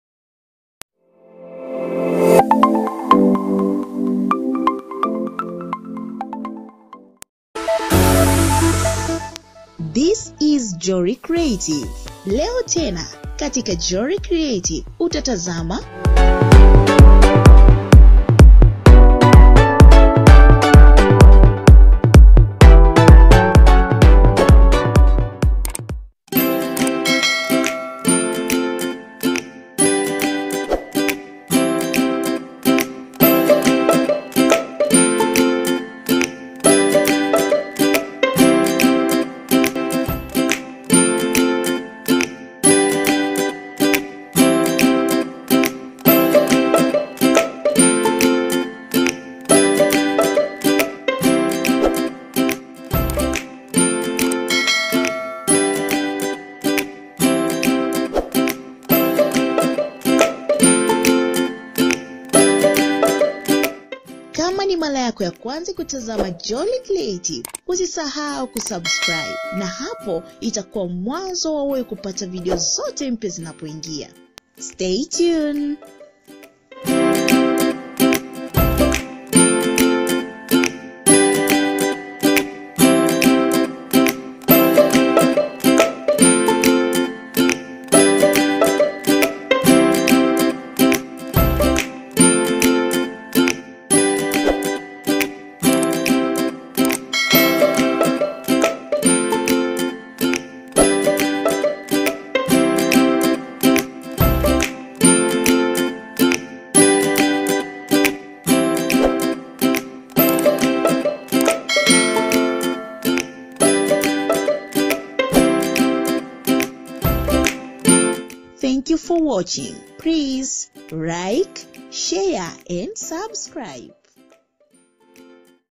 TOP 7 OF GIRLS/ BOYS BEDROOM IDEAS FOR SMALL ROOMS.
Decorating your home can become more challenging when thinking up children’s bedroom ideas – satisfying both a grown-up artistic and a younger perspective on decor can result in two.
Boys' bedroom decor ideas, much like those for girls, are helped by having a theme that can be easily updated. Children love to have a narrative in their bedrooms in which they feel they’re the main character, be it intergalactic spaceman or intrepid explorer. Therefore, shaping their room’s inside around that theme makes it a much more exciting and enjoyable environment.
# BrooksLycris
# Santoz Tv
# Abelvil Tv
wasafi TV
# Ayo Tv